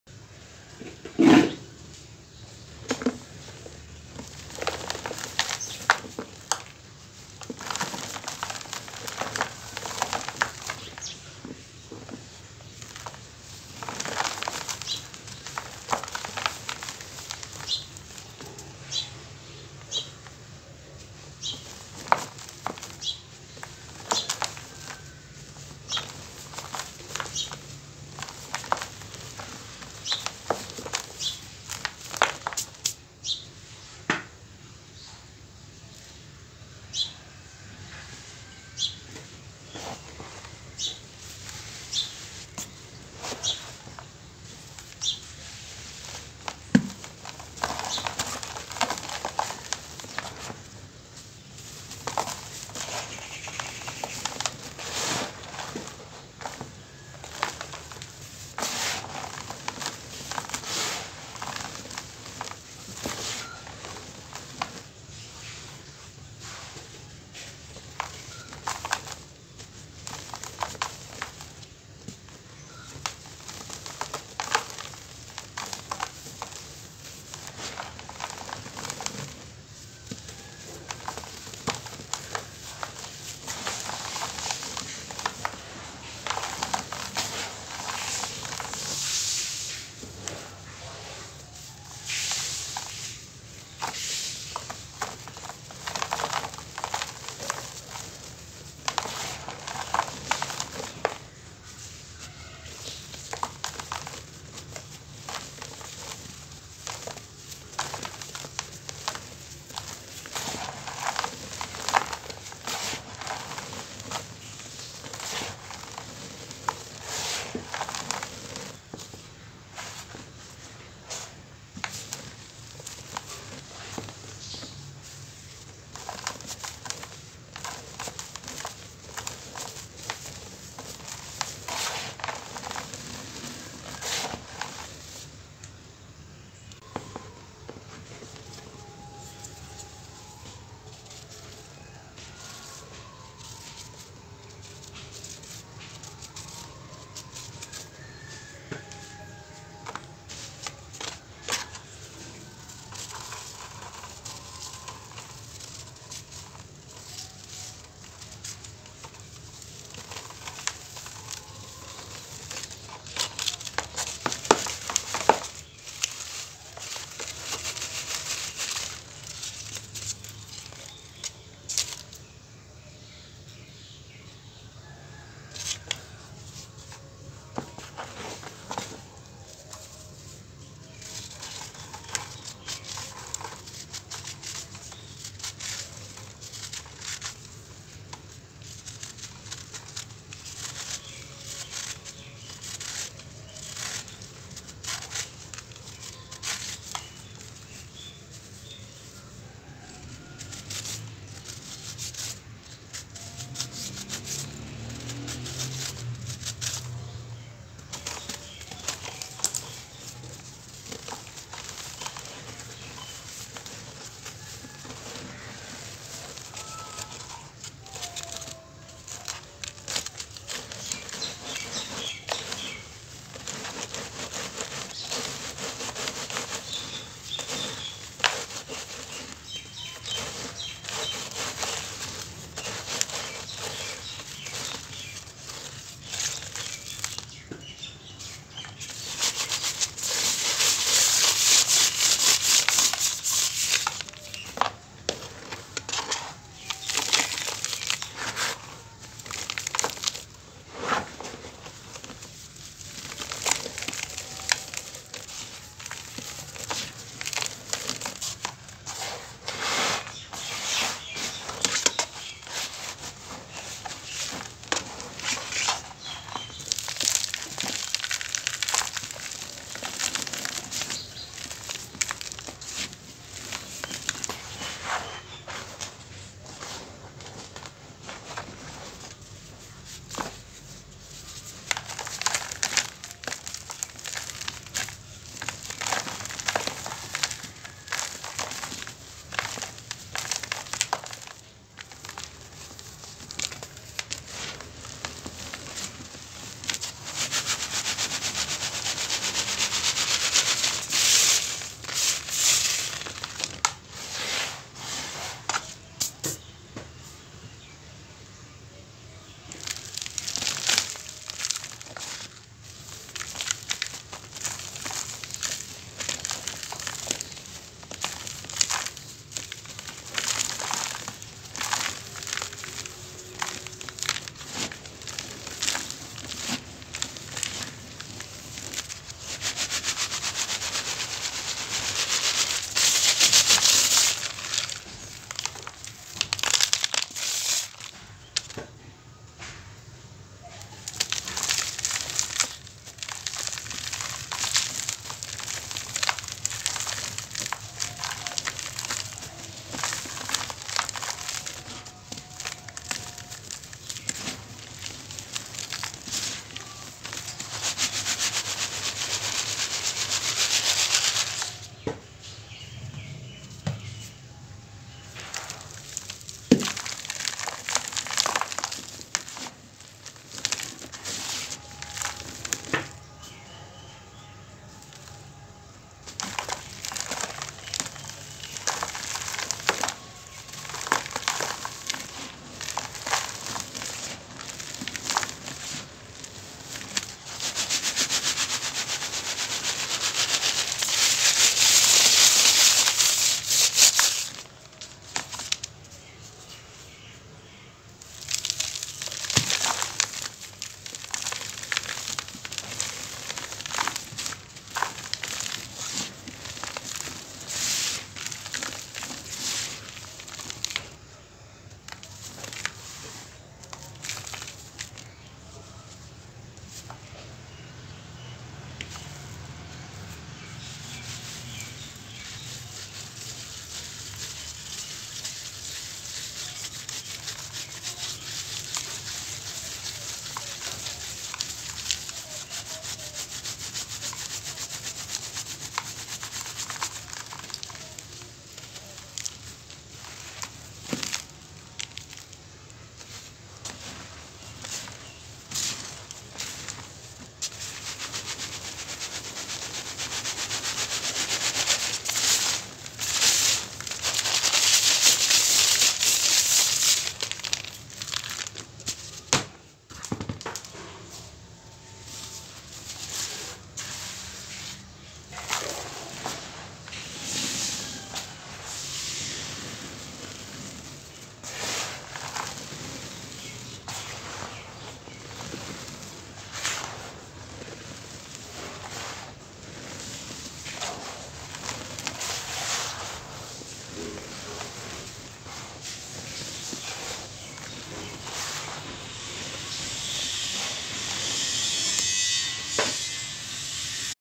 smokey and dusty 😜 pure cement crumble
This is an ASMR video that helps you to ..!!❤️❤️❤️

Pure Mud Crumbling In Water 💦 and paste play the most requested video..!!  🤎🤎🤎🤎Thanks for supporting me ..!!